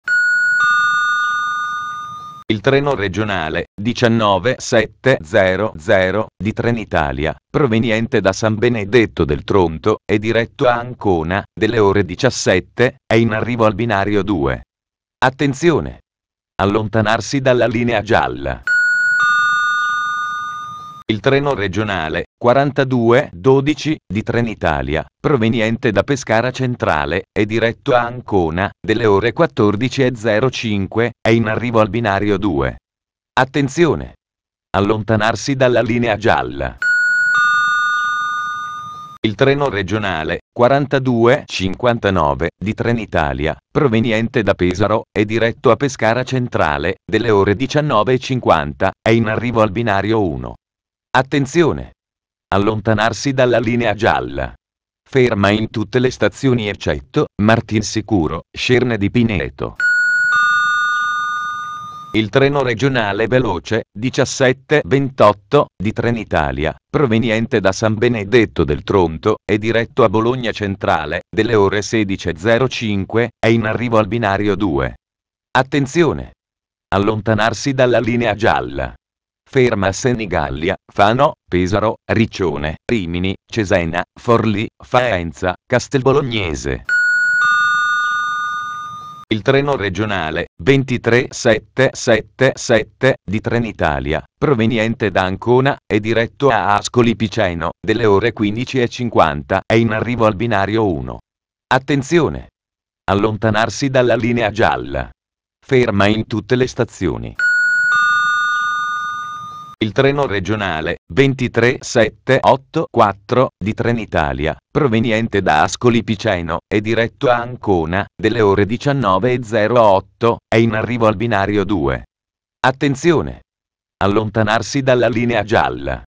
Annunci alla Stazione di Ancona Stadio
Oggi ci troviamo nella periferia della città di Ancona.

ANNUNCI - 2'
Italia, 2024